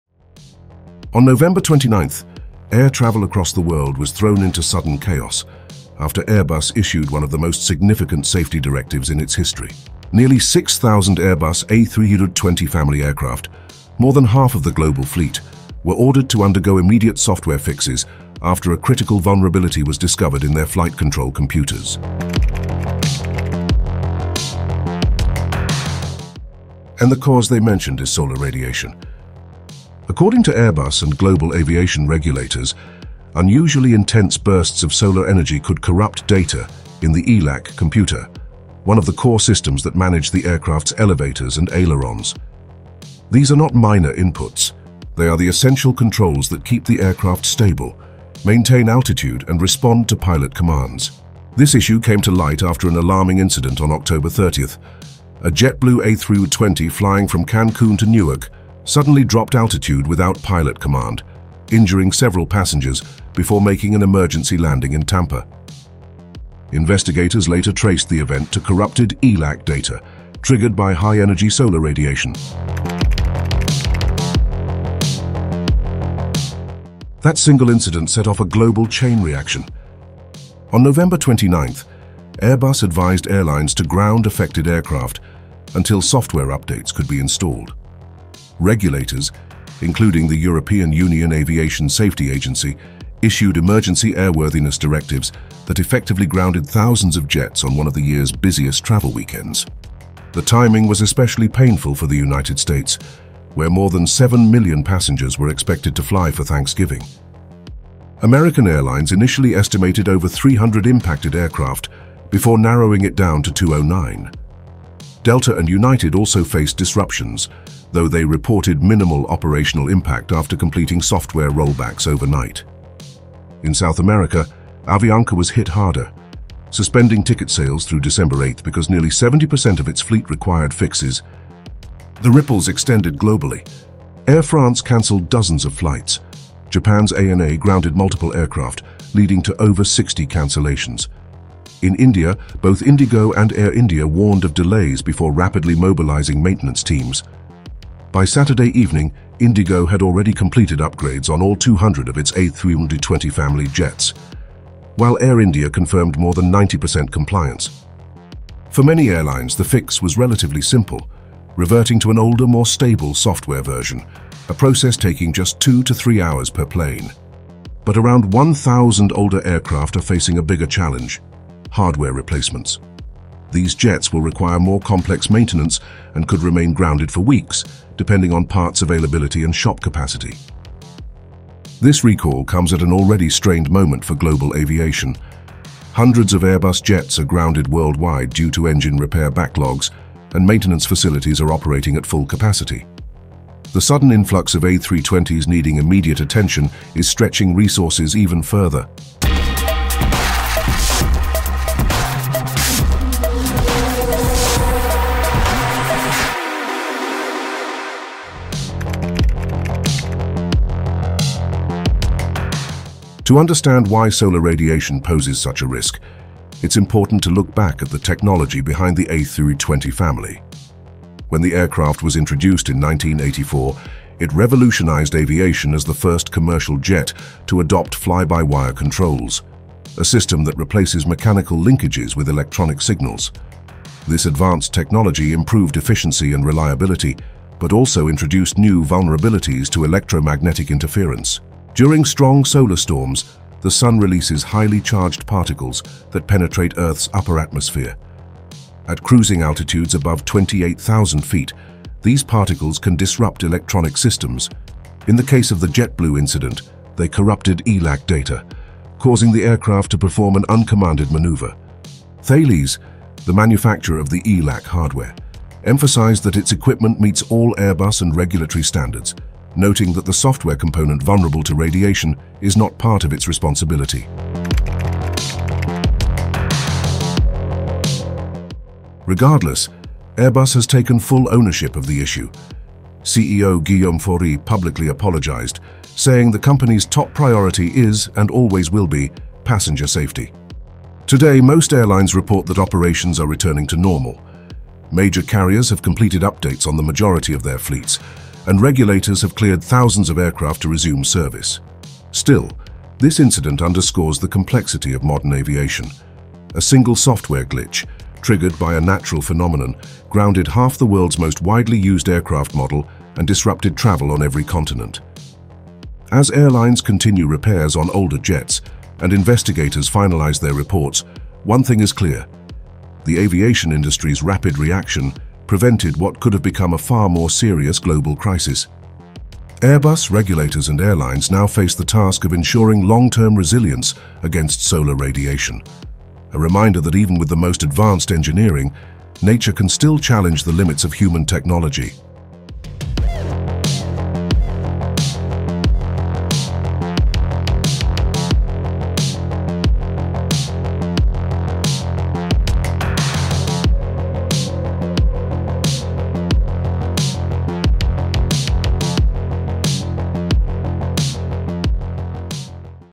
Airbus A320 Software Issue: How A ‘Solar Attack’ Scare Forced 6,000 Planes To Stay Grounded? Watch
Airbus A320 family faces largest grounding in its 55-year history after discovering intense solar radiation can corrupt flight-control computers, affecting 6,000 jets worldwide—over half the global A320 fleet. The crisis began with JetBlue Flight 1230's sudden uncommanded pitch-down at 35,000 feet (Oct 30), injuring 15 passengers, prompting EASA's emergency directive banning flights until Elevator Aileron Computer (ELAC) software L104 is fixed. Watch the video to know more details.